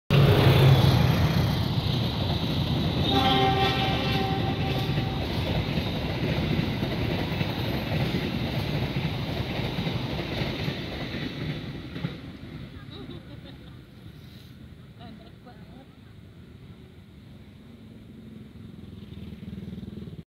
Kereta api Doble Kawis || Tiber stasiun Wates
kereta doble kawis melaju ditimur Stasiun Wates Pukul 17:00 WIB.

terimakasih